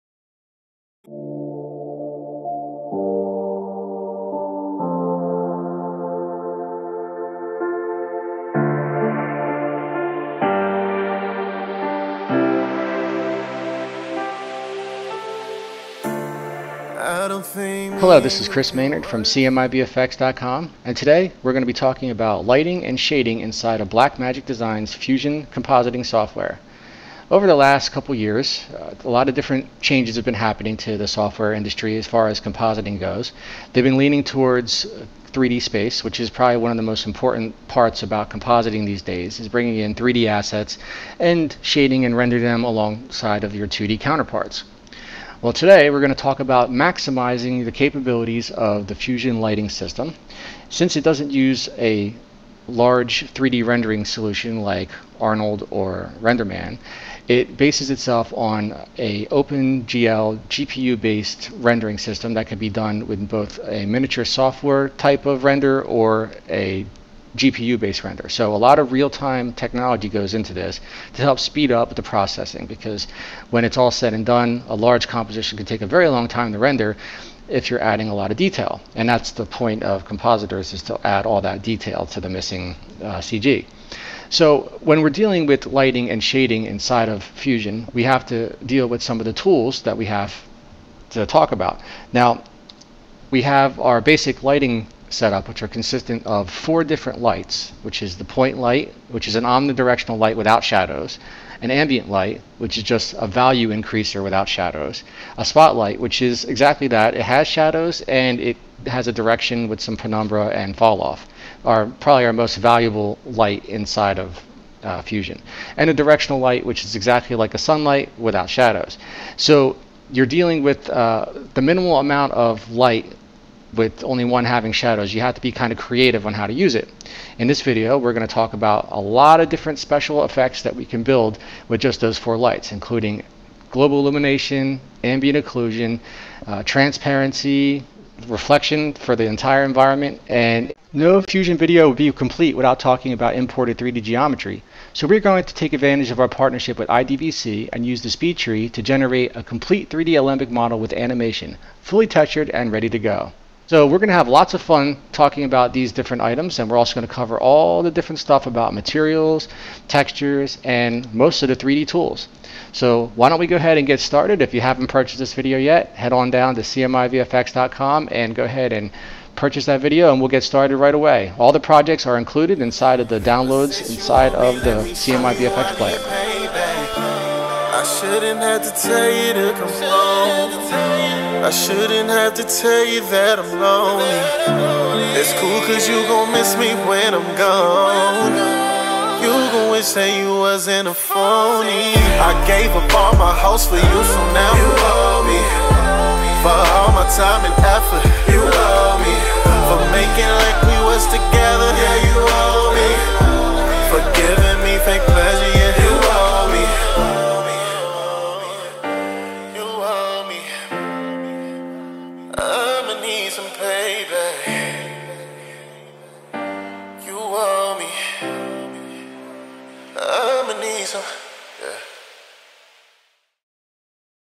cmiVFX Fusion Advanced Lighting
Fusion Advanced Lighting And Rendering Techniques
High Definition Training Videos for the Visual Effects Industry

Princeton, NJ (October 5, 2015) Getting excellent results in your Visual Effects compositing projects are just a few clicks away! No matter where you started in your career, you can now find a flavor of Blackmagic Design Fusion to fit your production pipeline needs. In this double feature-length Fusion training video, we show you what all OSX and Linux VFX Customers have been begging for. After so many years, Fusion is re-born with Unix support, and we take advantage of this by showing you what can be done for FREE on OSX in FUSION! The topics covered in this video range through, Lighting, Shading, Rendering, Fog, Alembic Animation, and Subsurface Scattering, Ambient Occlusion and Global Illumination simulation. We say “Simulation” simply because Fusion’s render engine relies heavily on cross platform OpenCL/Gl rendering which by default, lacks parameters for those 3 commonly sought after, CG Application settings. To achieve these very aggressive goals, we use our years of experience with Fusion to create reusable scenarios to help artists expand upon them in their commercial work. When it comes to innovation in the Visual Effects and Computer Graphics industries, there is only one choice… cmiVFX.com!

Short Description
Find out the Top Secret lighting techniques for Fusion in this killer Double Feature-Length Video with project files included!